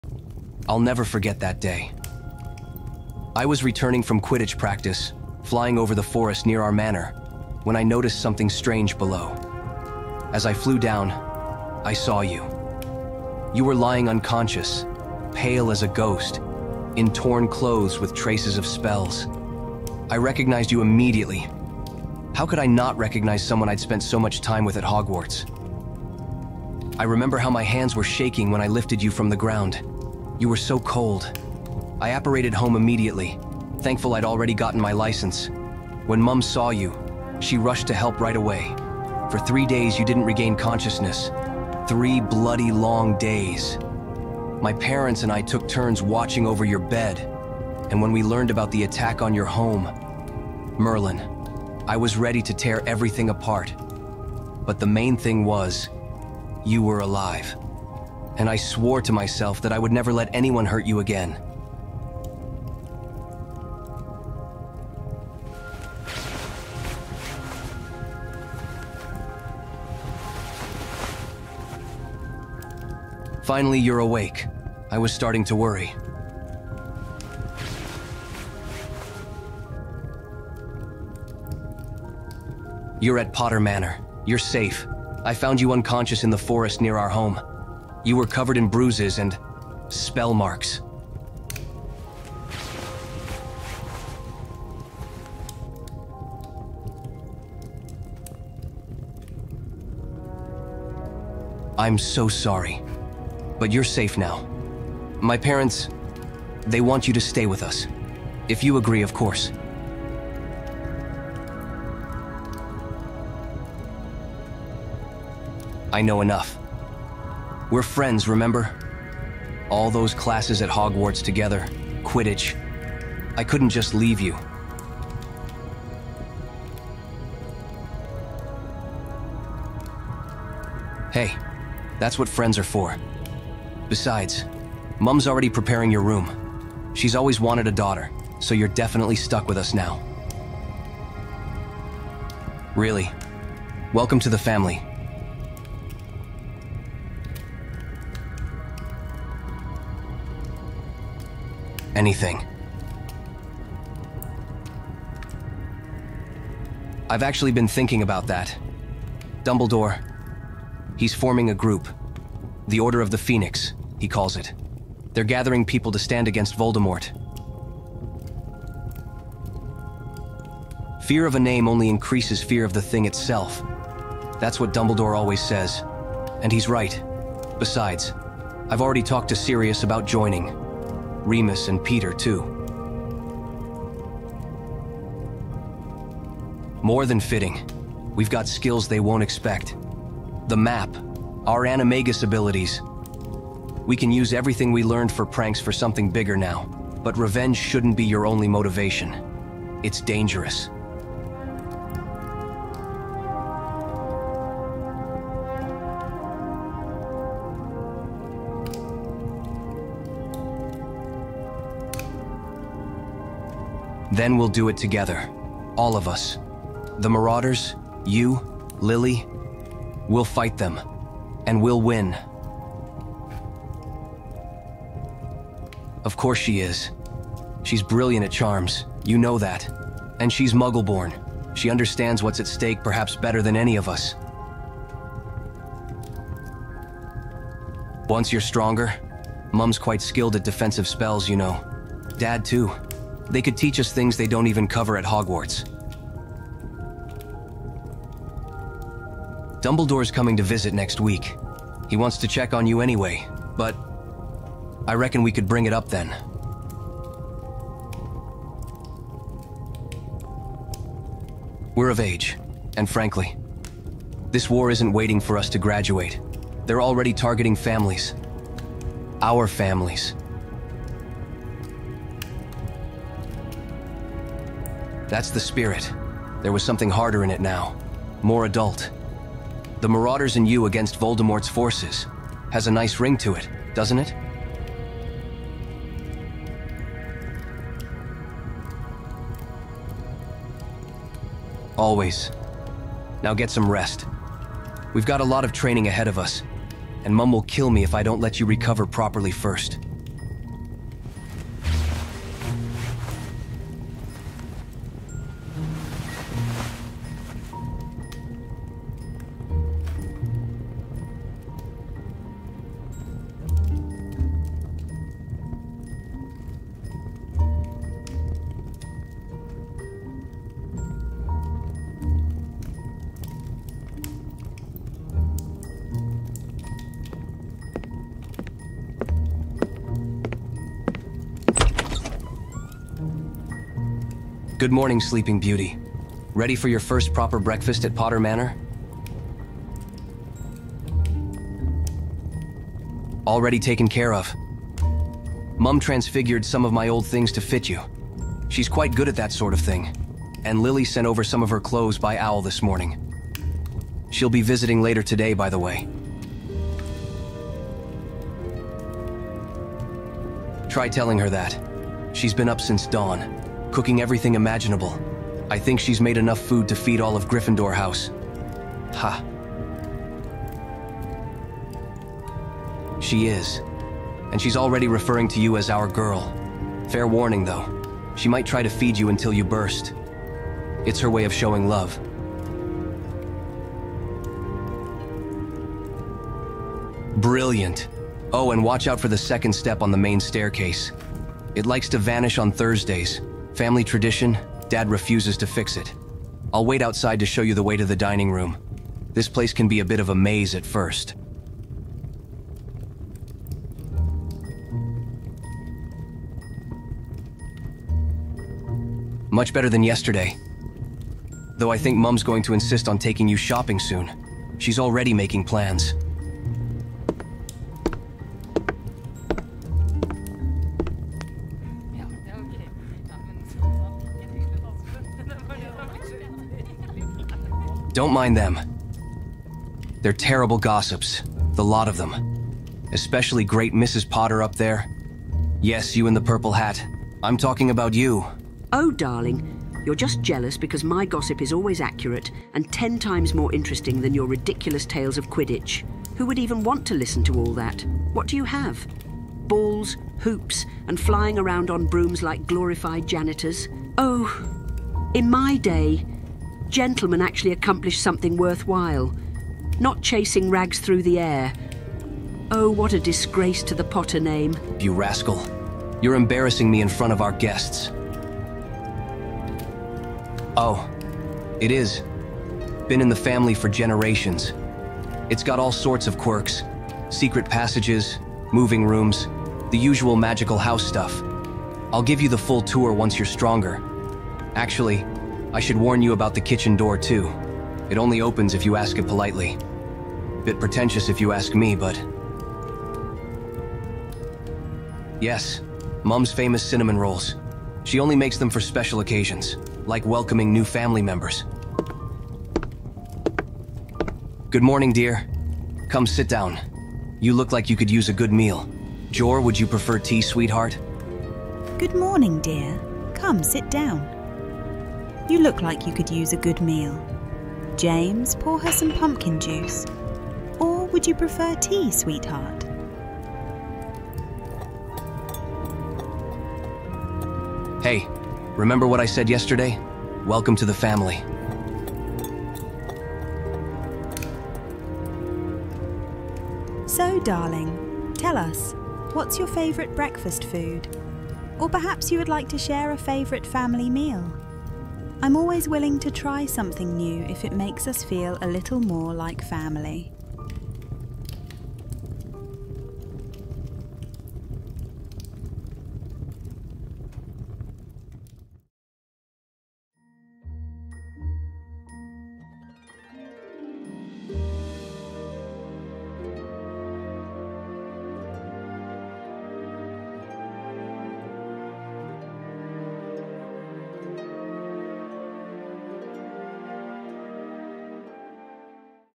James Potter decides to welcome you into his family - ASMR RP (comfort)
You can still hear the screams, the explosions shaking the house. Your parents told you to hide in the basement and not come out, no matter what happened. You hid there with your hands over your ears, trying to muffle the sounds of the attack from above. You were so scared, so helpless.
When the noise finally stopped, you realised you had to get out of there. You pushed open the cellar door and ran, ran as fast as you could through the woods surrounding our house. You didn't know where you were going-you just had to get out of there, as far away as you could from the horror you were witnessing.
You didn't know how long you were unconscious, but when you finally came to, you were in an unfamiliar room. Sitting by your my bed was James Potter, looking at you with such kindness and concern.
James and his family, the Potters, found you and took you in this Christmas holidays making you happier. After all, you now have a family after all the grief you've been through.